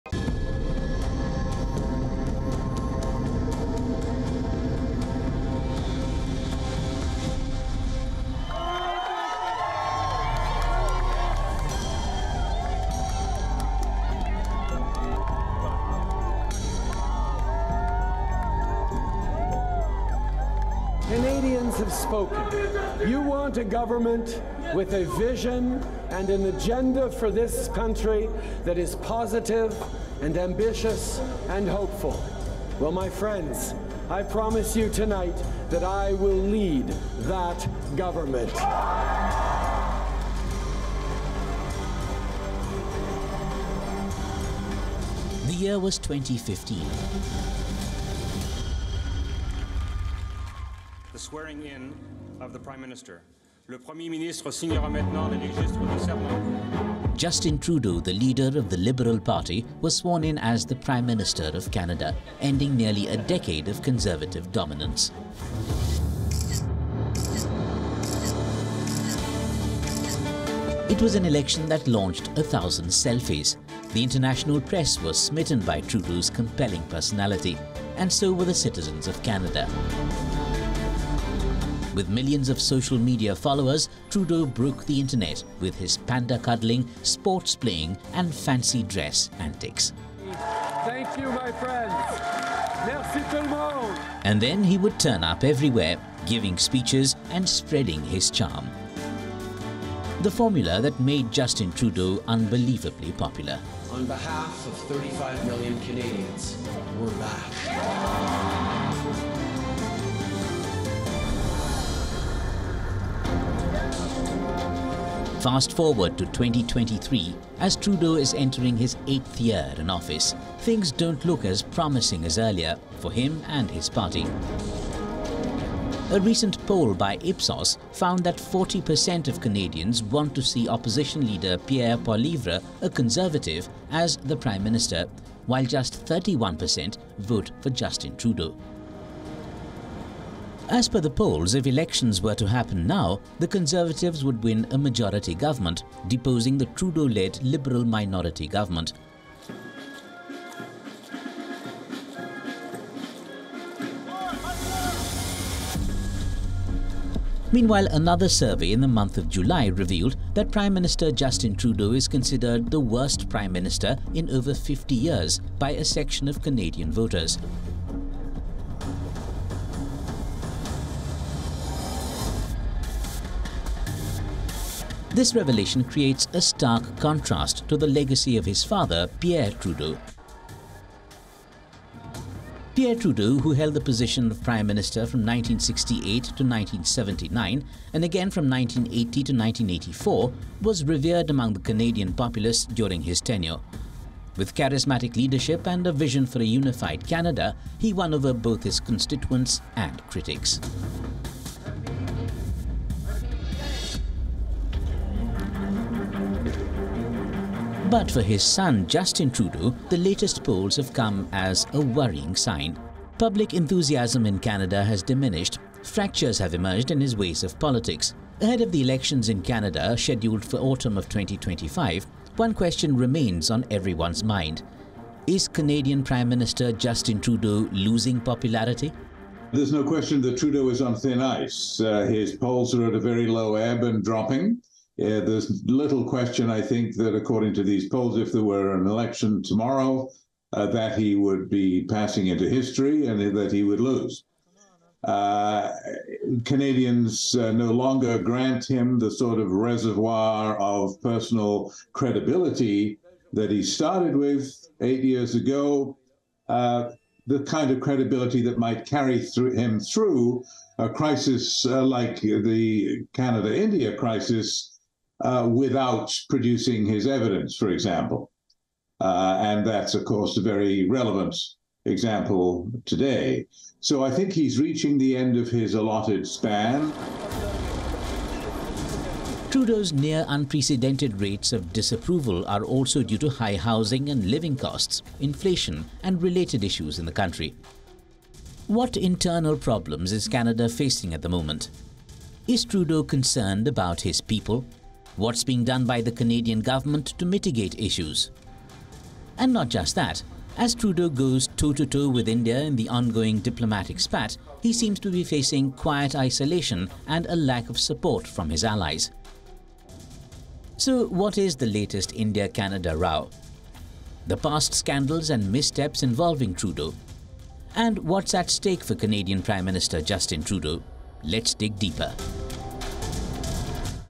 Is Canada's Trudeau losing popularity? | WION Wideangle
When Justin Trudeau became Canada's Prime Minister for the first time back in 2015, his popularity was soaring high. The international media as well as the citizens of Canada were smitten by his personality. However, eight years down the line, Trudeau's popularity seems to be fading. According to a recent survey, the once popular poster boy has now been voted the worst Canadian Prime Minister in the last 50 years. Why has the liberal icon fallen from grace?